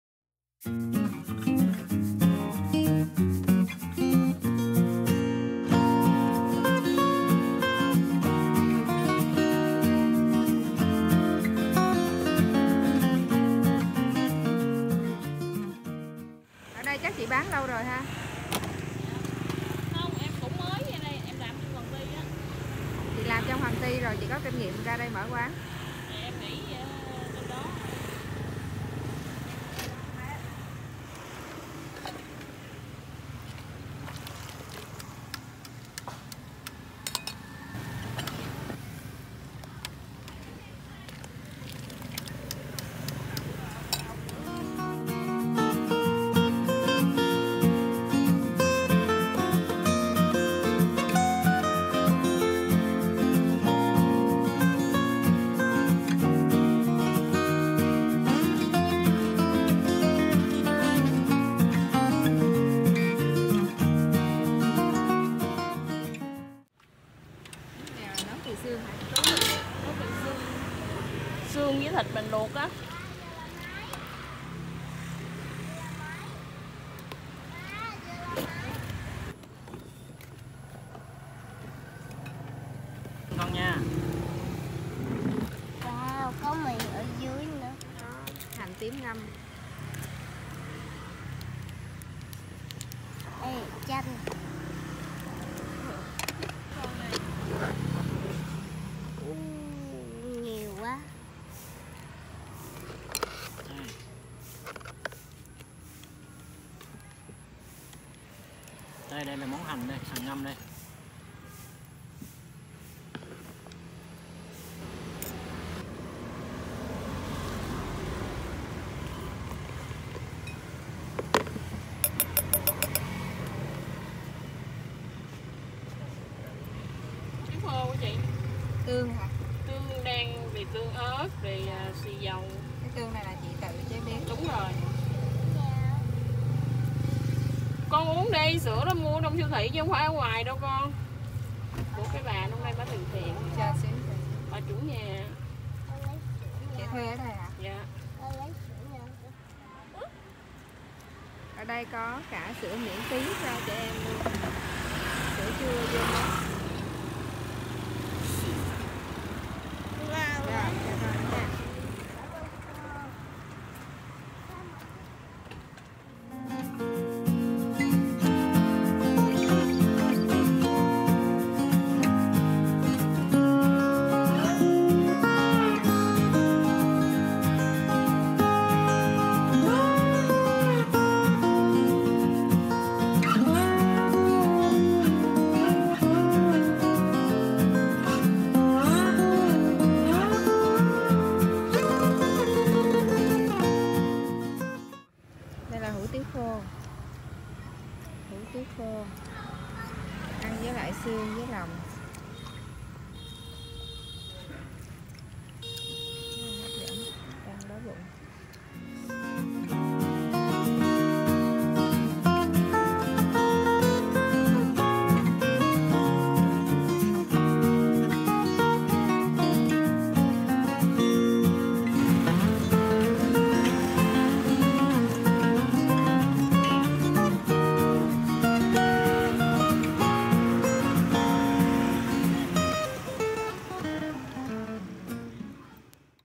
Mê ly với món hủ tiếu lòng bán ở vỉa hè Thanh Đa- Guufood
Tại vỉa hè đường Bình Quới, đối diện với khu du lịch Bình Quới, ở bán đảo Thanh Đa, quận Bình Thạnh, có 1 quán hủ tiếu lòng heo cực ngon. Chị chủ quán cho biết, quán mở không bao lâu nhưng khá đông khách qua lại ghé vào thưởng thức.

Lòng heo chị làm món hủ tiếu, mỳ này được chế biến sạch sẽ, đảm bảo vệ sinh. Do đó, khi thực khách đã 1 lần ăn tại quán hủ tiếu lòng vỉa hè này thì chắc chắn sẽ quay lại.

Còn chờ gì nữa các bạn, nếu có dịp đi ngang thì nhớ ghé đến ủng hộ chị chủ quán nhé!!!